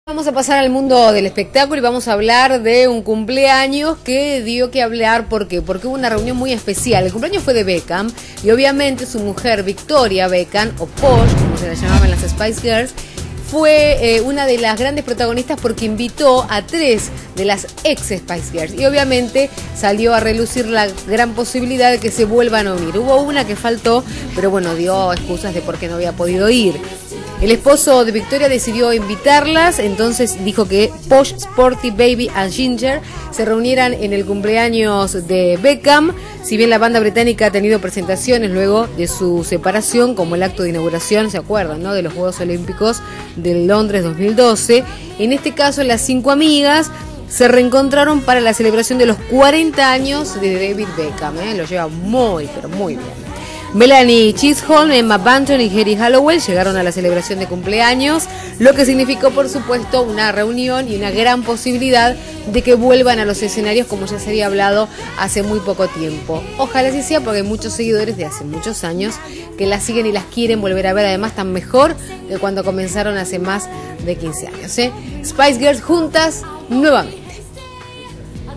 Canal 26 -Spice Girls tuvieron reencuentro en cumpleaños de David Beckham
Las ex integrantes del desaparecido grupo de pop Spice Girls tuvieron un emotivo reencuentro en Marruecos, país donde David Beckham celebró su cumpleaños número 40.